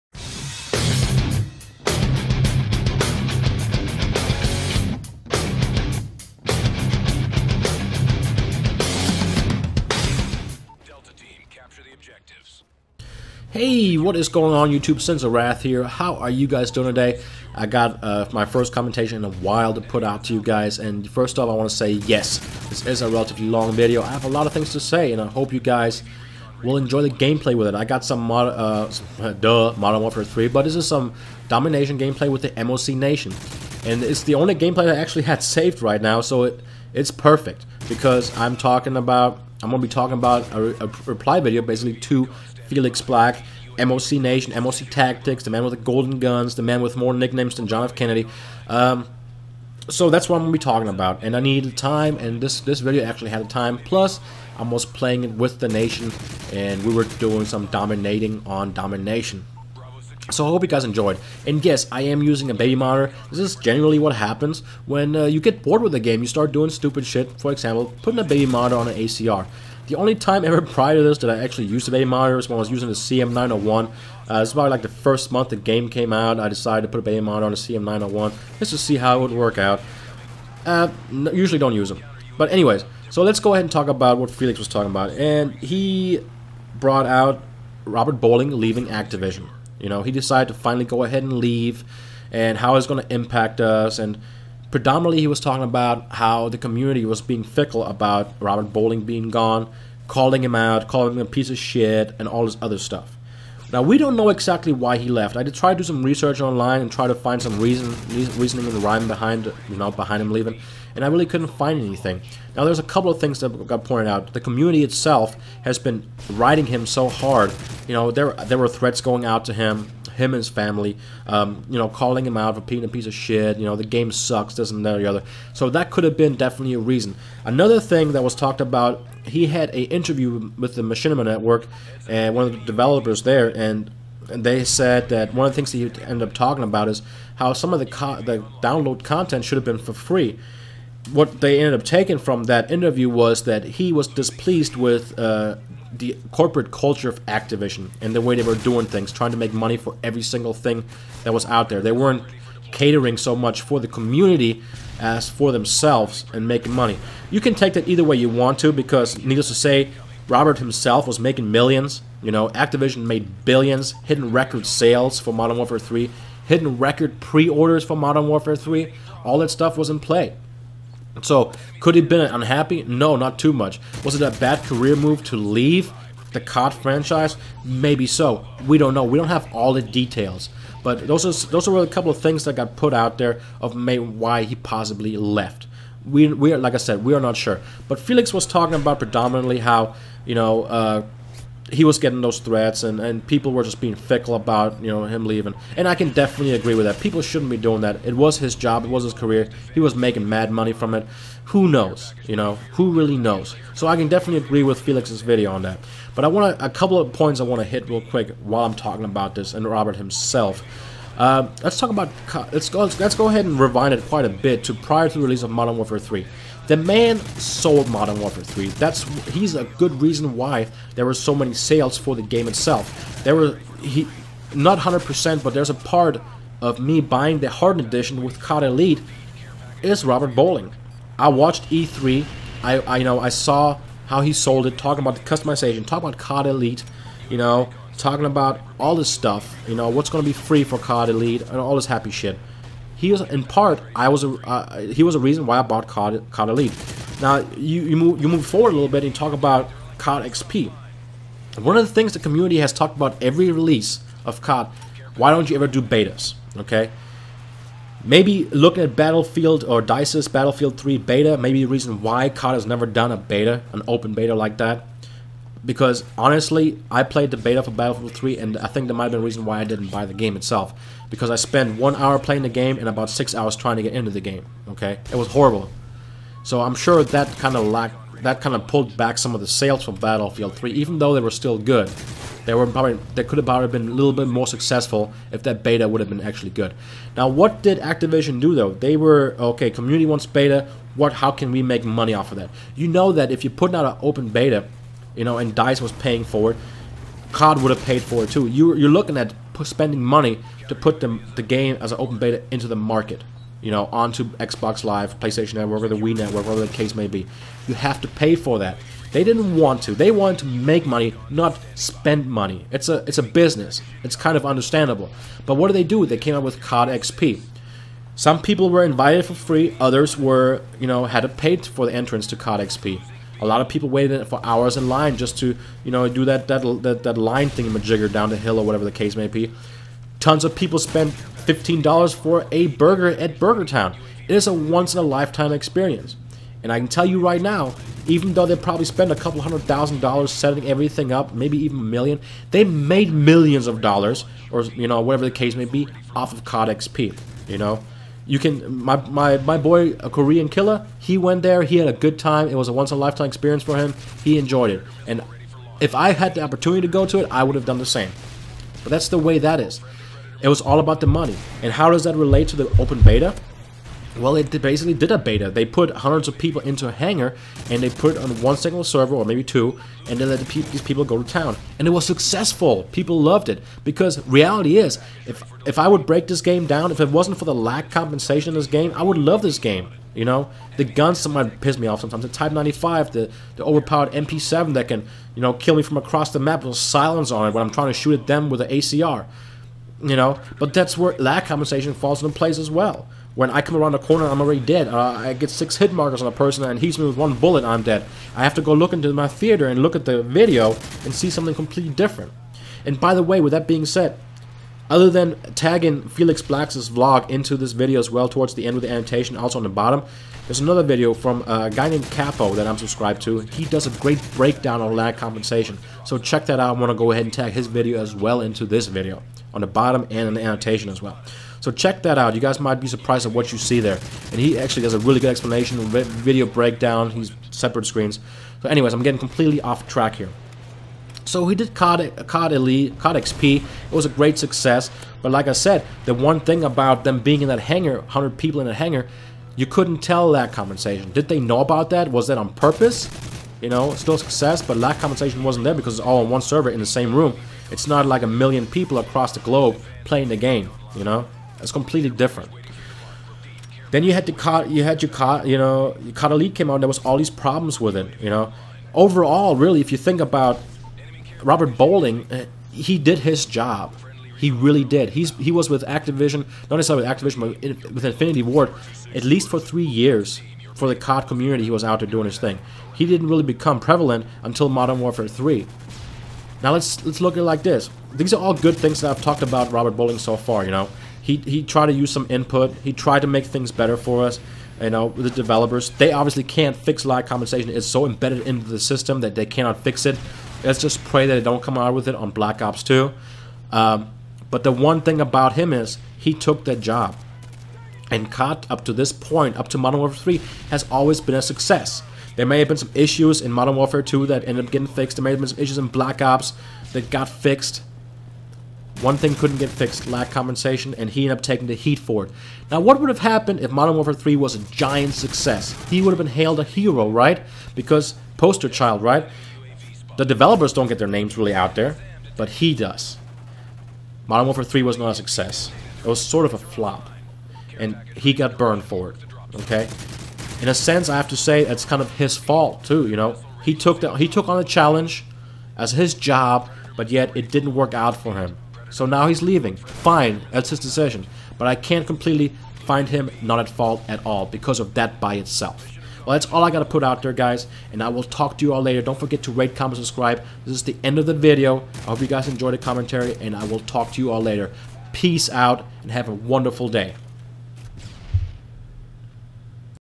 Reply to MoCTactics | Robert Bowling departing Activision
My reply to MoCTactics, xFelix Blackx video in regards to Robert Bowling leaving Activision.